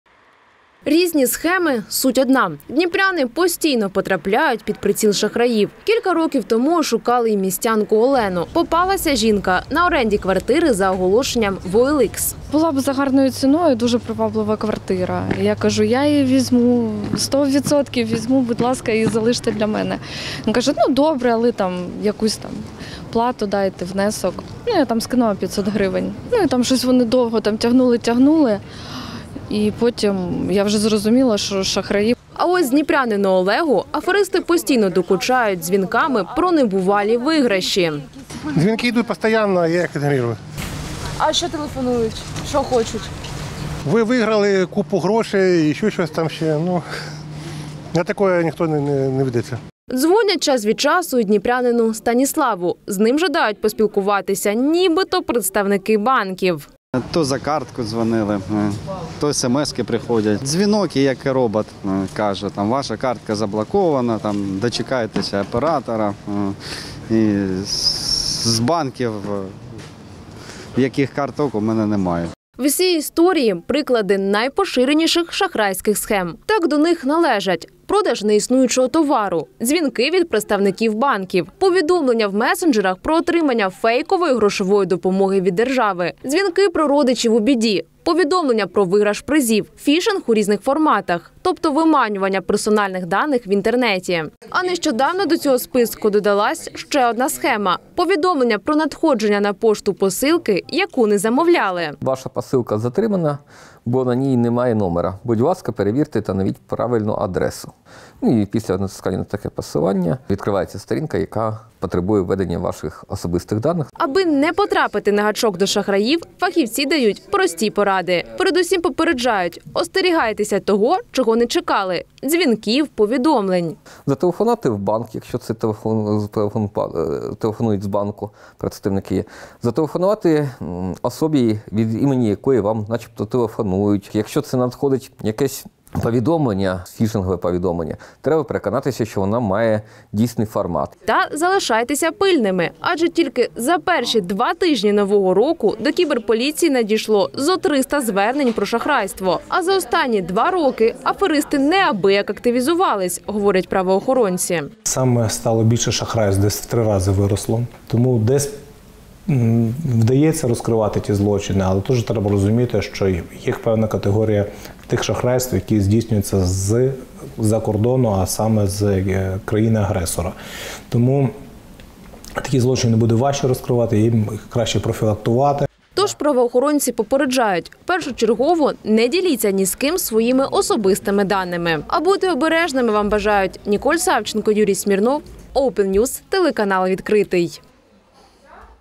Втричі більше: у Дніпрі зросла кількість шахрайств
Втричі більше. За час повномасштабного вторгнення зросла кількість шахрайств. Гроші виманюють у дзвінках, повідомленнях, інтернет-оголошеннях. Які найпоширеніші схеми? Та що знову вигадали аферисти?